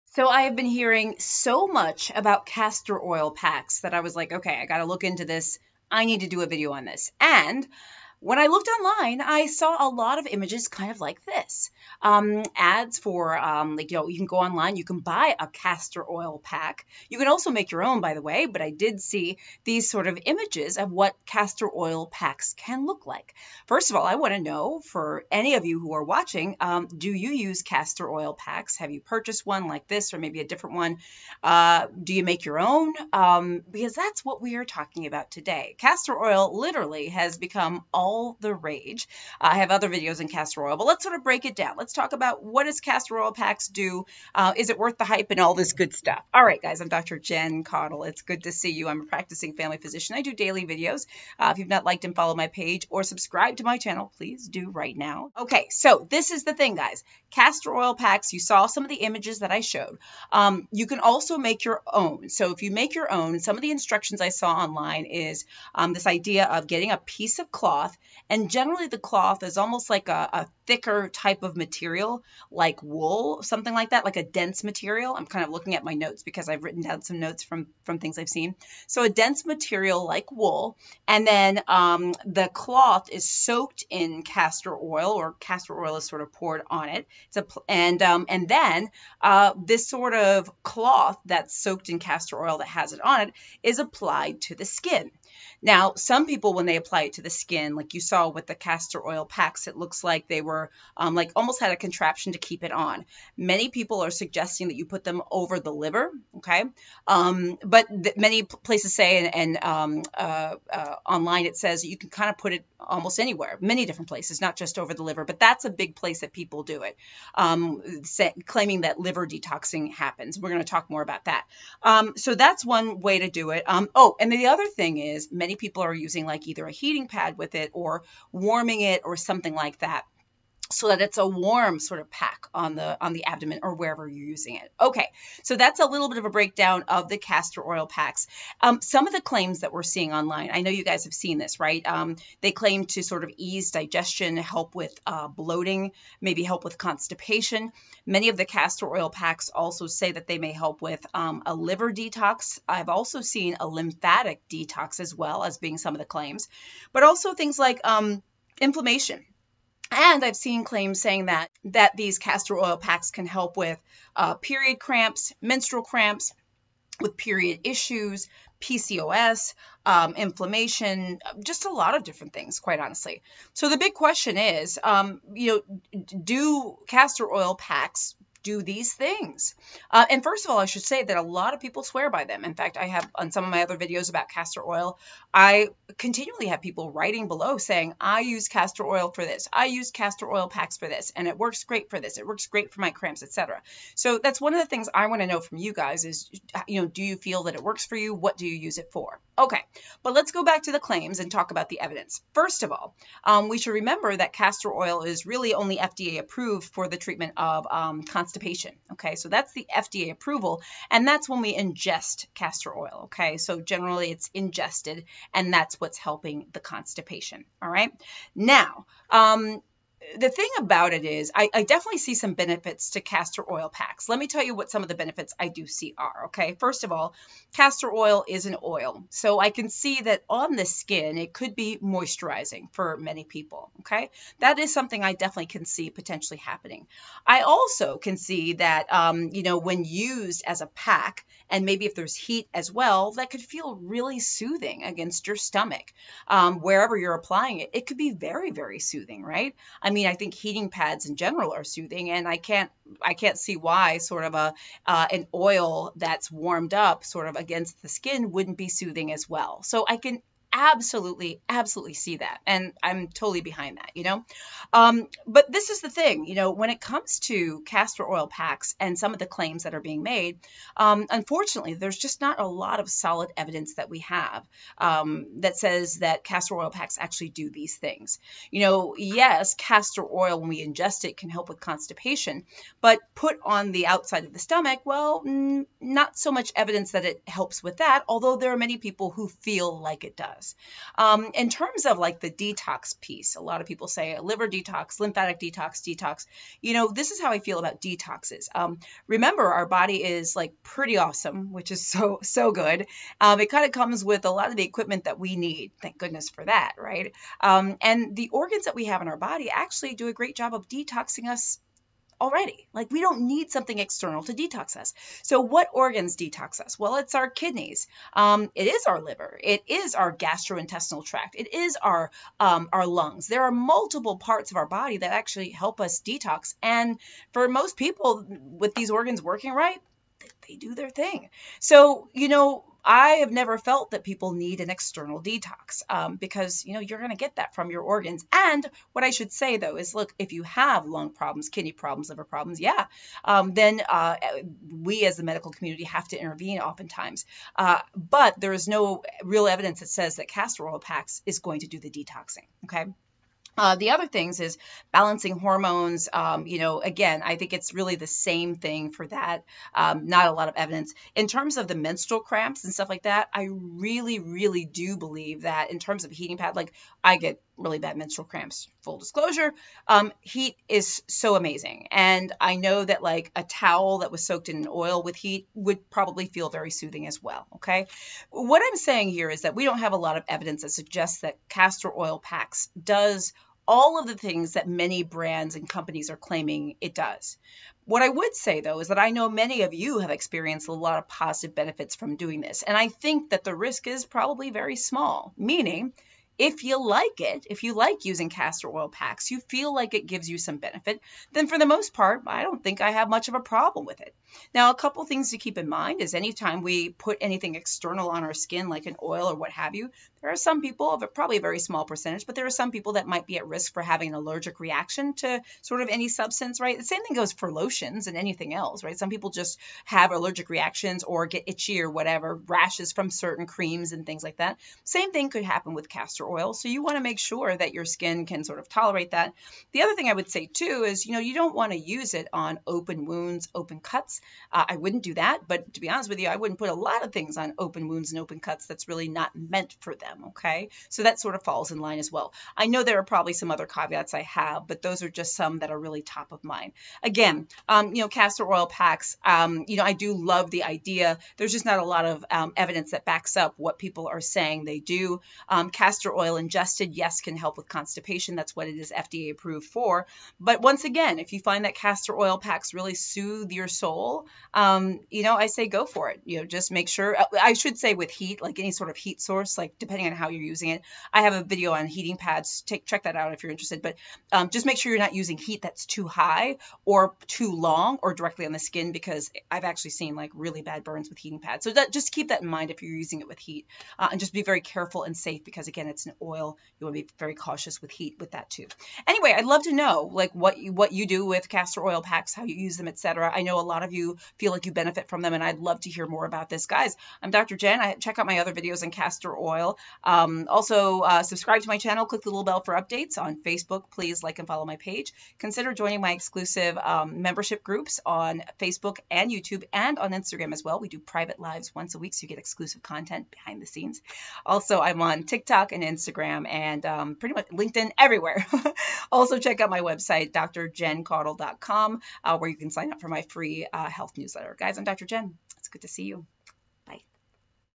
Castor Oil Packs: Do They Detox (Liver, Lymphatics), Help Digestion, Menstruation & Immunity etc.?
Do Castor Oil Packs Help with Digestion, Detoxing, Menstruation, Inflammation and more?